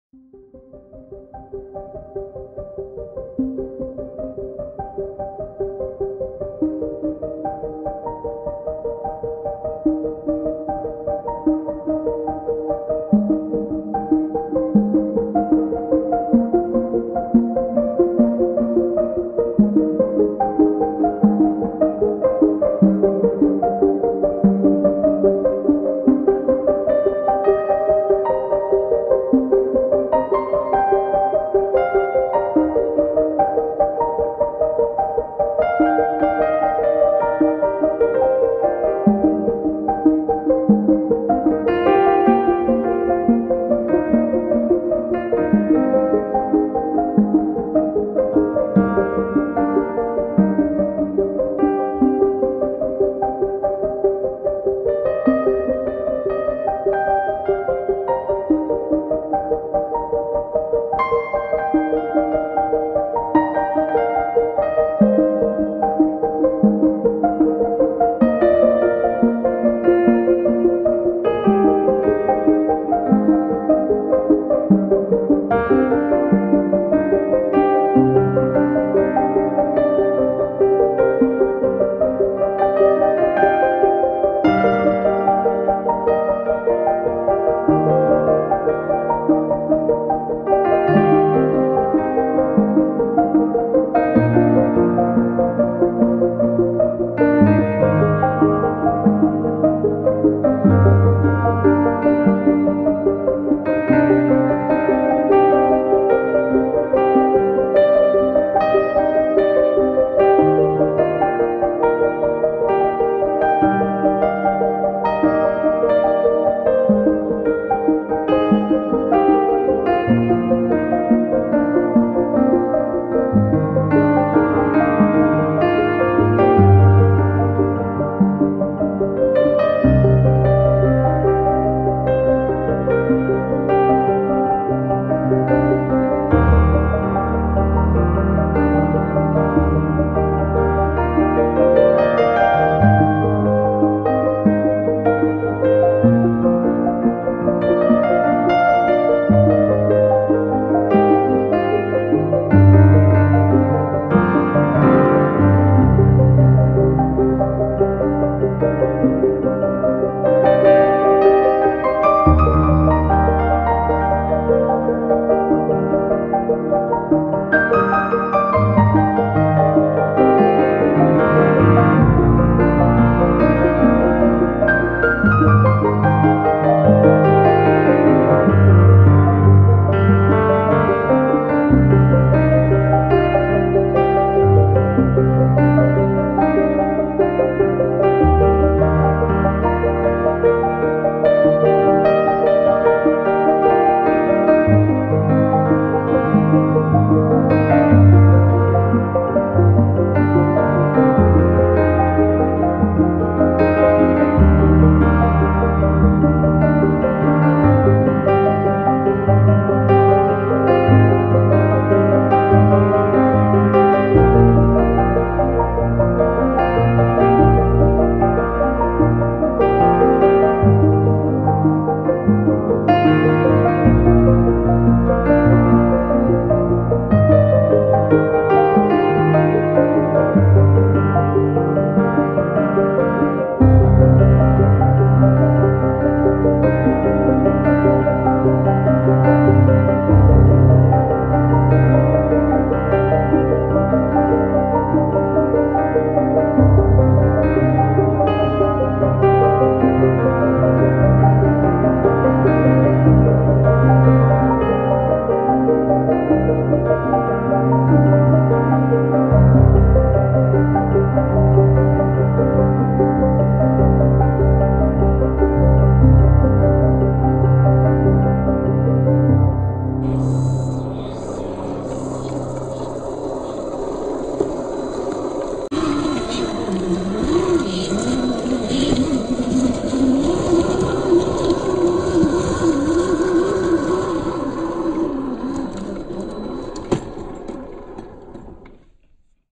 RC : Jeep Wrangler Rubicon JK Off-road Trail #16
camera : iphone XS
Body : Xtra speed Jeep hard body V2
chassis : T-rock(SCX10 II Clone)
wheel : gmade SR04
tire : 1.9" voodoo